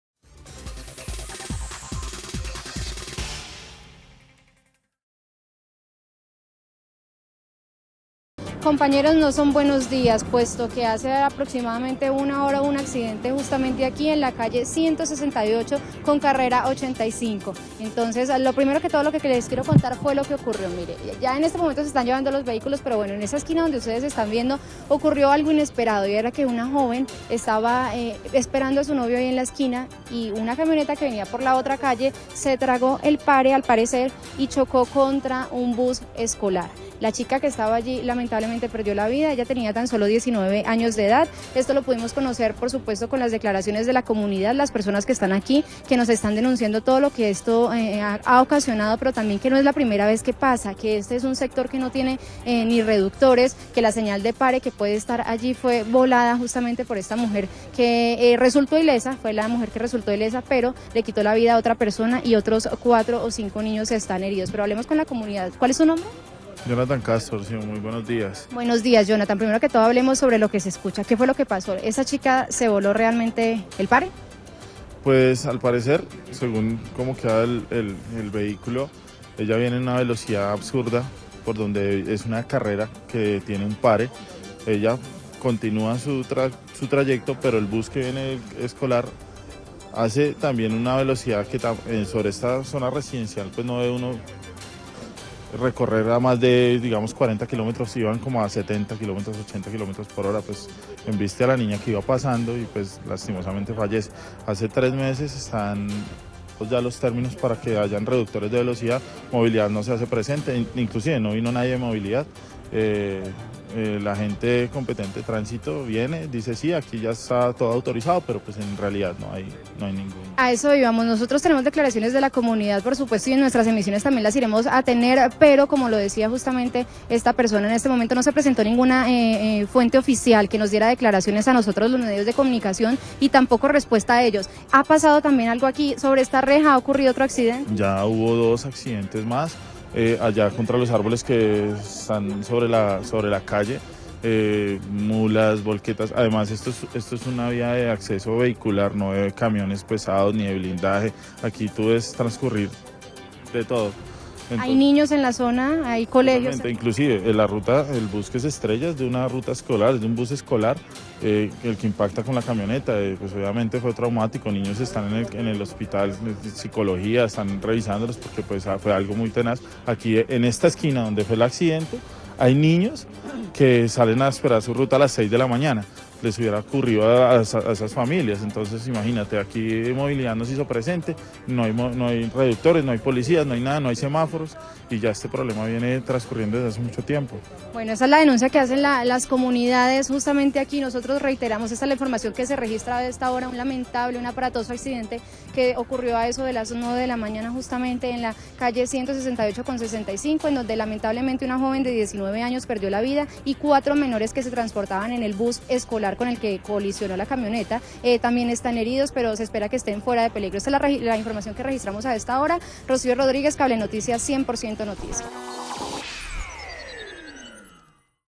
Un muerto y cuatro heridos tras choque de vehículo y ruta escolar en Bogotá
En la mañana de este martes en Bogotá, se registró un fuerte choque entre una ruta escolar y un vehículo particular, el fuerte accidente dejó una joven muerta y cuatro menores heridos.